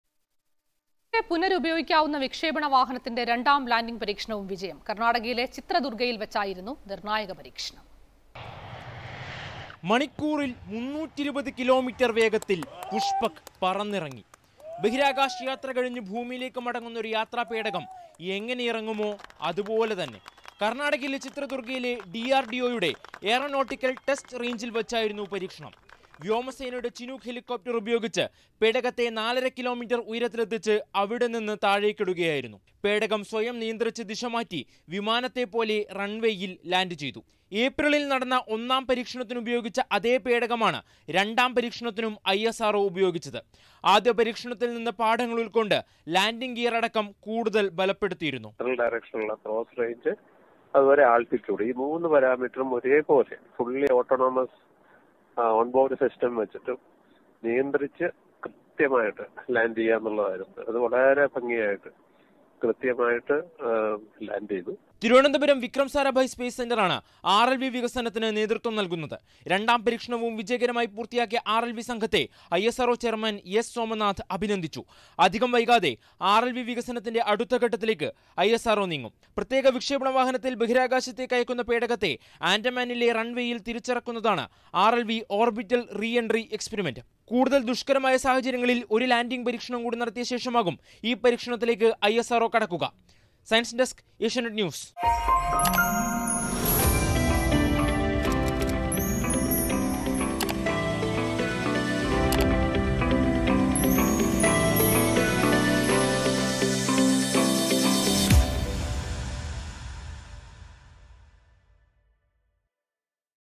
RLVയുടെ രണ്ടാം ലാൻഡിങ് പരീക്ഷണവും വിജയം
RLVയുടെ രണ്ടാം ലാൻഡിങ് പരീക്ഷണവും വിജയം  ; പേടകം സ്വയം നിയന്ത്രിച്ച് ദിശമാറ്റി വിമാനം പോലെ ലാൻഡ് ചെയ്തു